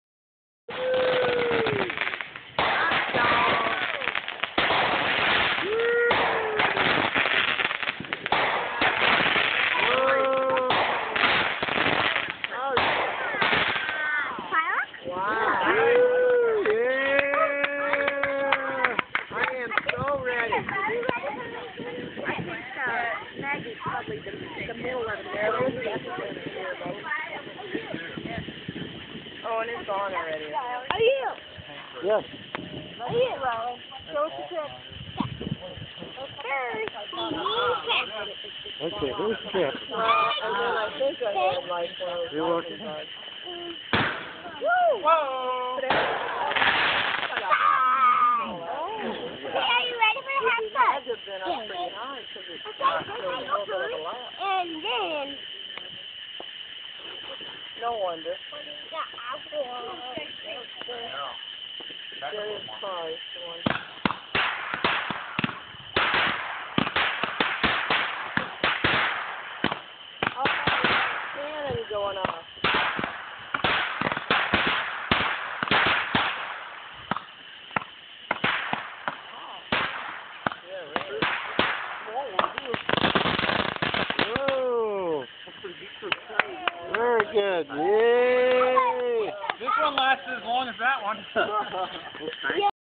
July 4, 2010 9:17 PM
July 4th, 2010 Fireworks
 at Shannon's parents place Ste.Genevieve Missouri